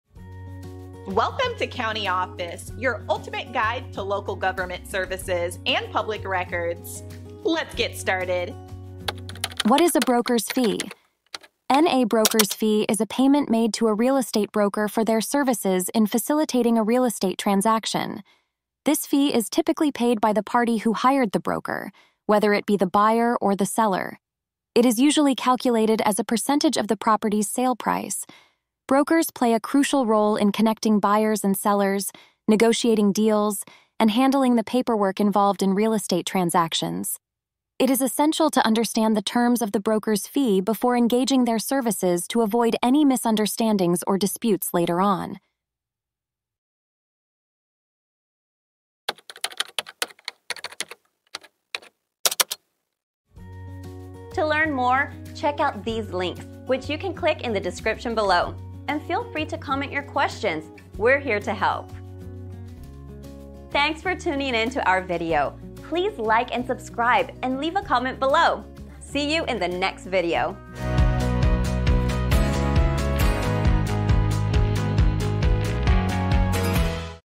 What Is A Brokers Fee? - CountyOffice.org
What Is A Brokers Fee? In this informative video, we'll delve into the topic of broker's fees in real estate transactions. Brokers play a vital role in connecting buyers and sellers, negotiating deals, and handling the necessary paperwork. Understanding the terms of a broker's fee is crucial to avoid misunderstandings or disputes down the line. Learn about what a broker's fee entails, who typically pays it, and how it is calculated based on the property's sale price.